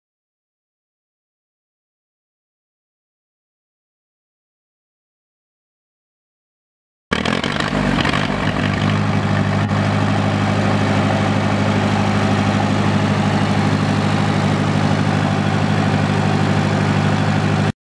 Lincoln SA200 welder for sale | sold at auction August 6, 2014
Lincoln SA200 welder
Four cylinder gas engine
2" ball hitch
25&#039; ground and electro leads included
205/75R14 tires
Serial A898003

It was located in Osage City, KS, and we sold it to the highest bidder for $1,760.